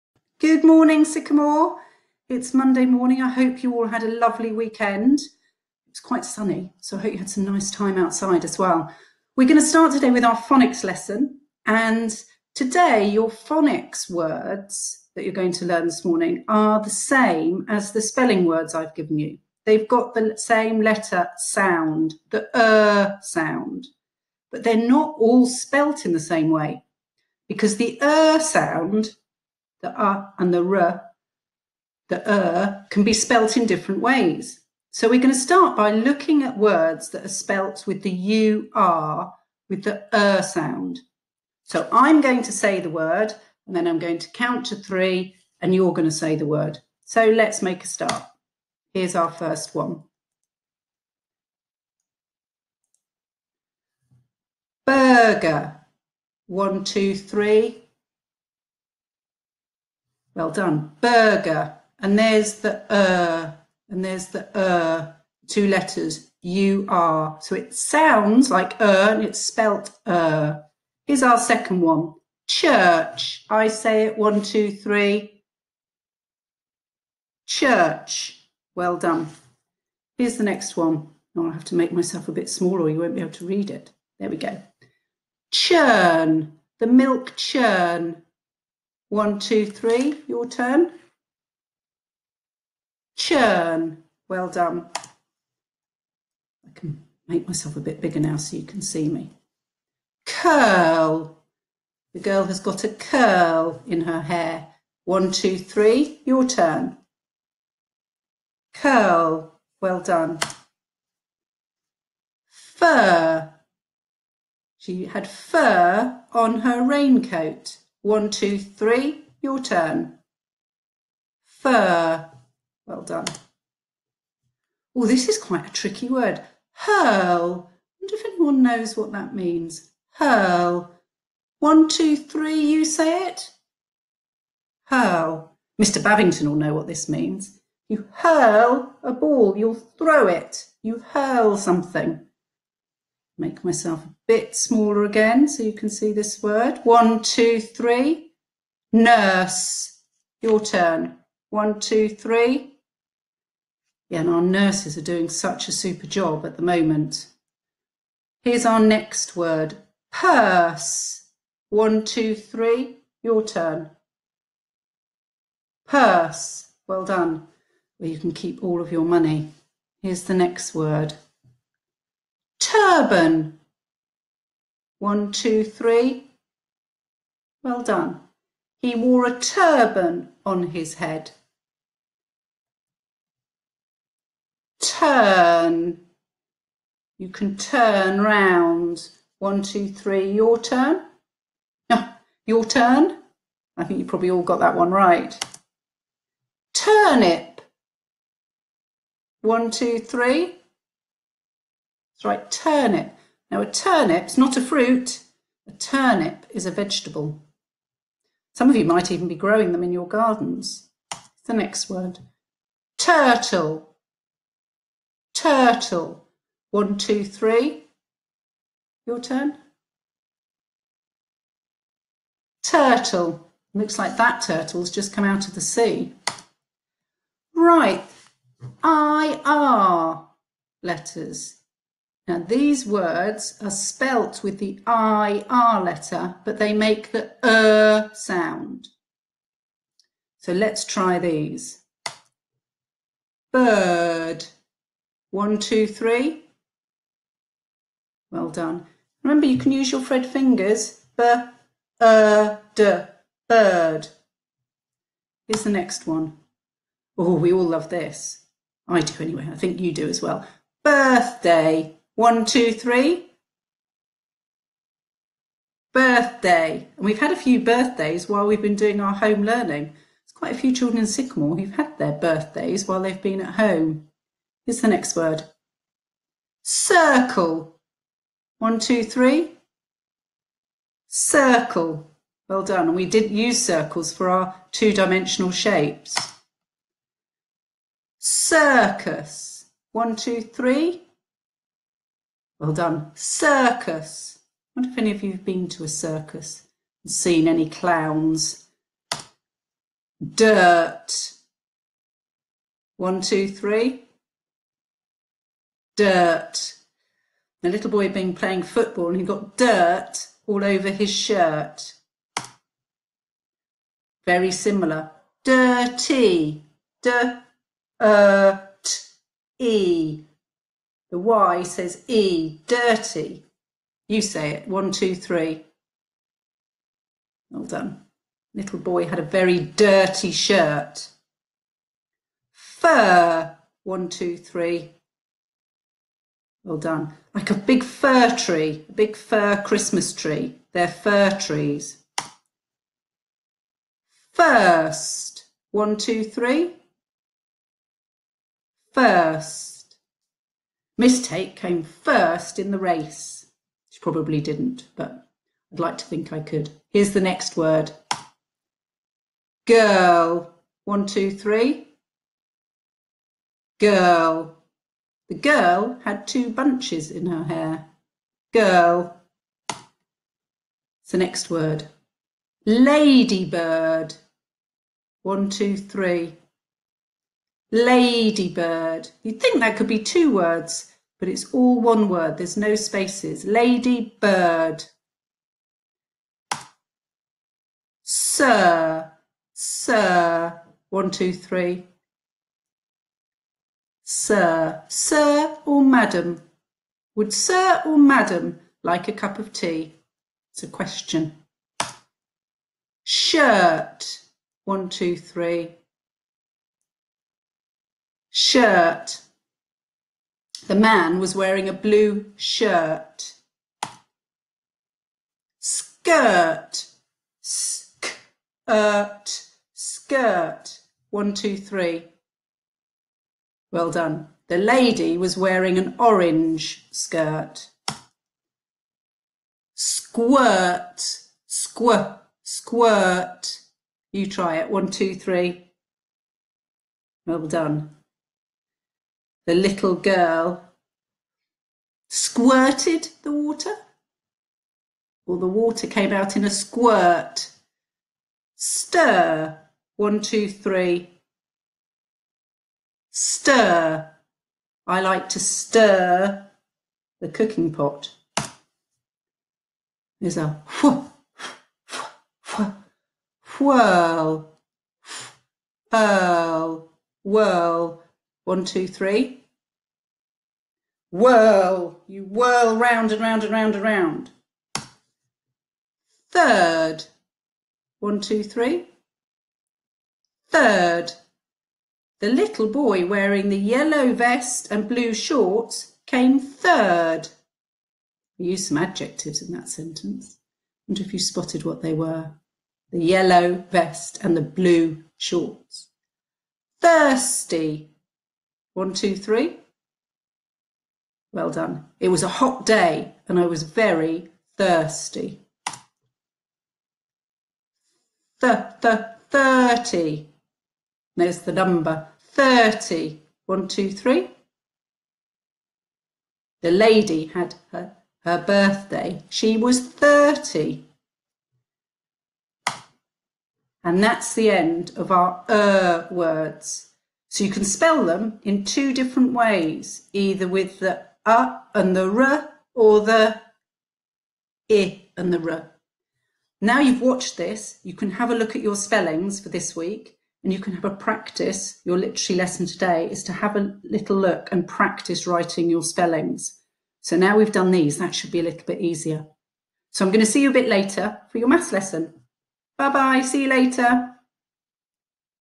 Sycamore Phonics lesson Monday 27th April
"ur" sound